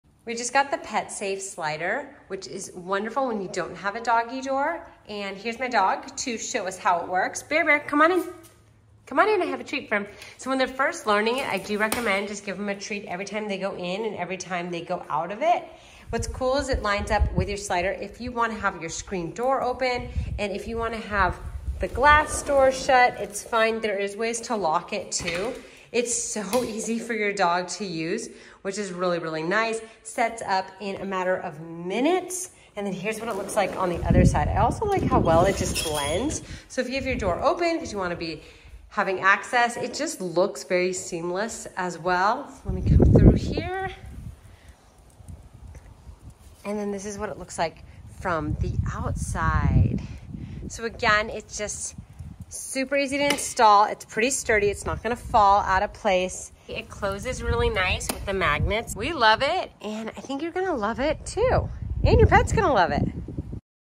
PetSafe 1-Piece Sliding Glass Pet Door Review
PetSafe 1-Piece Sliding Glass Pet Door for Dogs & Cats- Adjustable Height 75 7/8" to 81"-Medium, White, No-Cut DIY Install Aluminum Patio Panel Insert, Great for Renters or Seasonal Installation

Instead of cutting into a wall to building a dog door, this a wonderful option!
Our 25 pound American Eskimo uses the medium size door.

Discover the ultimate solution for pet owners with the PetSafe 1-Piece Sliding Glass Pet Door for Dogs & Cats. This innovative door seamlessly integrates with your sliding glass door, providing a hassle-free and secure entry for your furry friends. Our demo and review will uncover its features, benefits, and overall performance, ensuring you make an informed decision for your beloved pets.